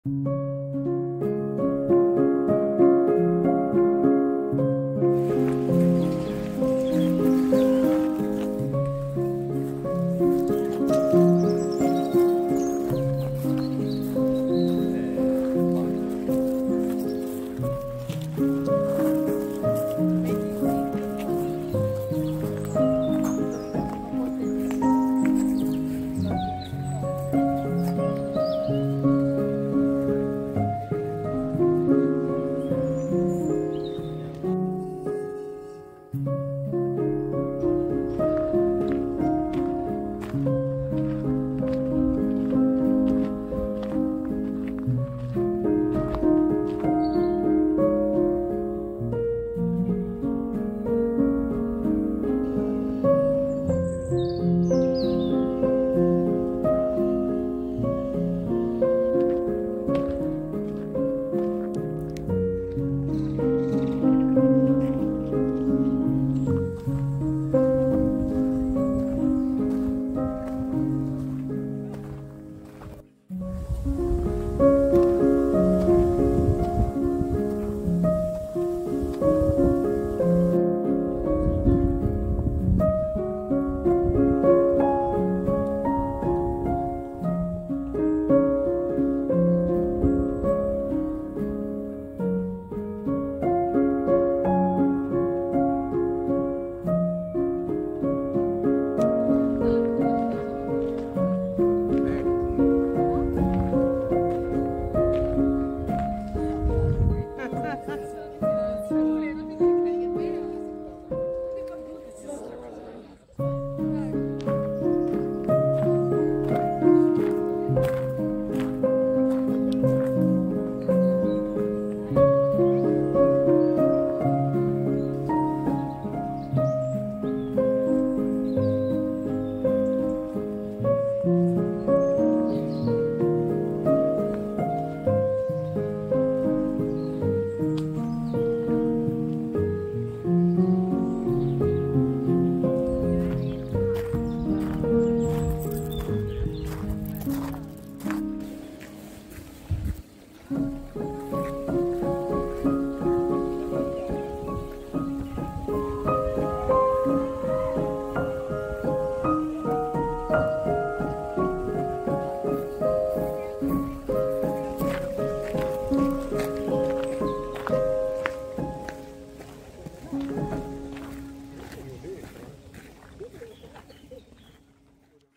Visit Florence - Stroll in Giardino di Boboli
Florence: promenade dans les jardins de Boboli (attenants au Palais Pitti).
Firenze: passeggiata nel Giardino di Boboli (adiacente a Palazzo Pitti).